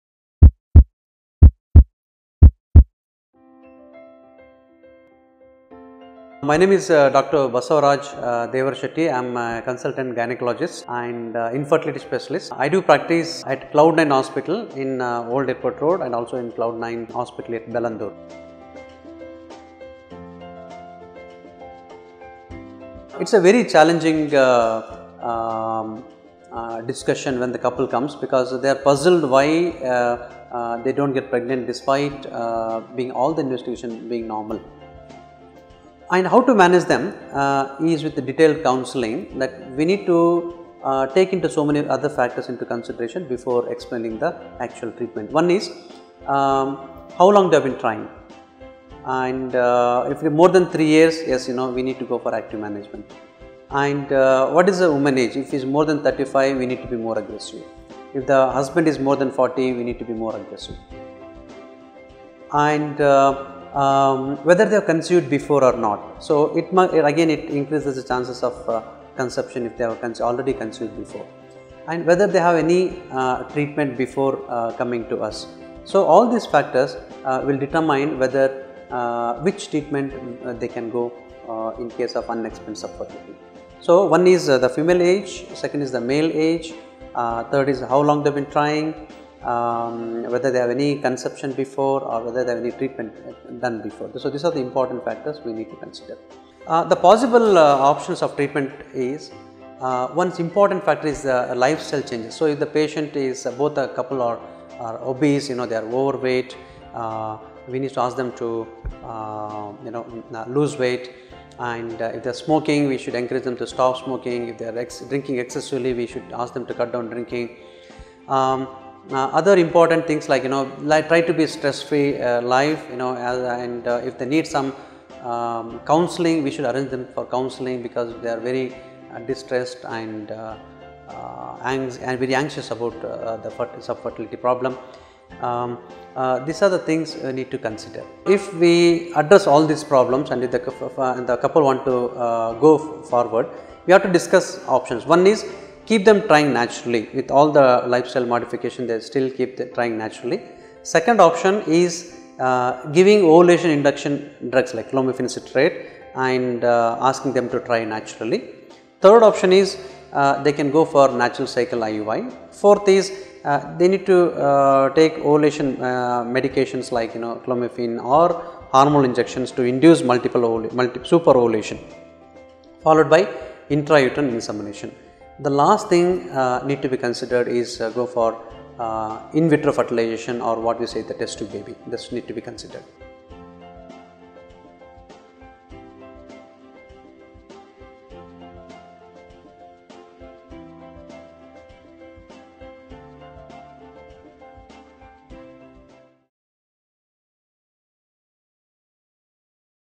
Tips to treat Unexplained Subfertility -Dr.Basavaraj Devarashetty of Cloudnine Hospitals
It is a very challenging discussion when the couples come because they are puzzled why they don’t get pregnant despite all the investigations being normal. How to manage them is through detailed counselling. We need to take into consideration so many other factors before explain the actual treatment. One is how long they have been trying. If it is more than 3 years we need to go for active management. What is the woman age? If she is more than 35 we need to be more aggressive. If the husband is more than 40 we need to be more aggressive and find whether they have conceived before or not. Again it increases the chances of conception if they have already conceived before. Whether they had any treatment before coming to us. All these factors determine which treatment they can go in case of unexplained infertility. These are the important factors we need to consider. Possible treatment is lifestyle changes. So if the couple is obese or they are over weight we need to ask them to lose weight and we should encourage them to stop smoking and cut down drinking. Try to live stress free life and if they need some counselling arrange them for counselling because they are very distressed and anxious about the fertility problem. These are the things we need to consider. If we address all these problems and the couple want to go forward we have to discuss options. One is keep them trying naturally with all the lifestyle modifications. Second option is giving ovulation induction drugs like clomiphene citrate and asking them to try naturally. Third option is they can go for natural cycle ivf. Fourth is they need to take ovulation medications like clomiphene or hormonal injections to induce super ovulation followed by intrauterine insemination. The last thing you need to consider is go for in vitro fertilization or what you say test tube baby.